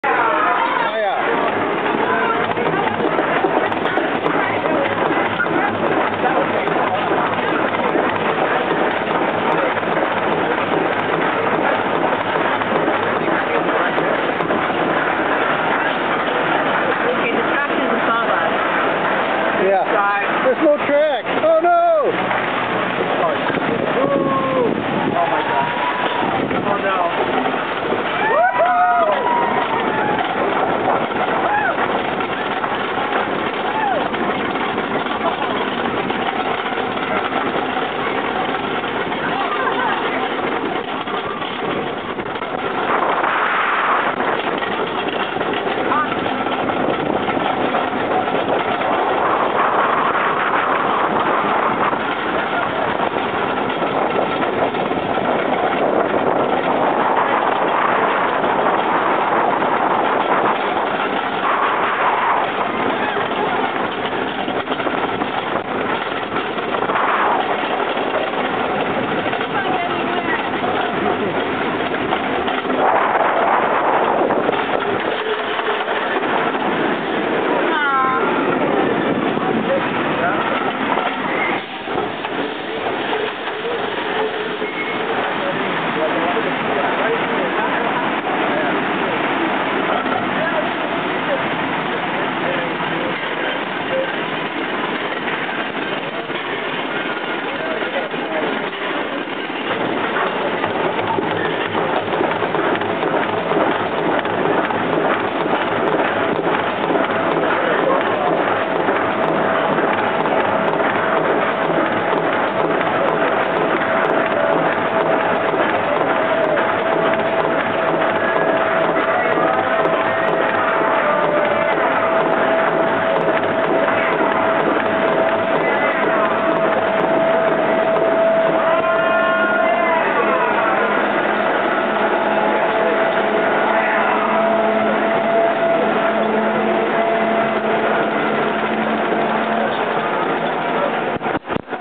Magic Mountain Ninja Roller Coaster POV
Here we ride the Magic Mountain Ninja.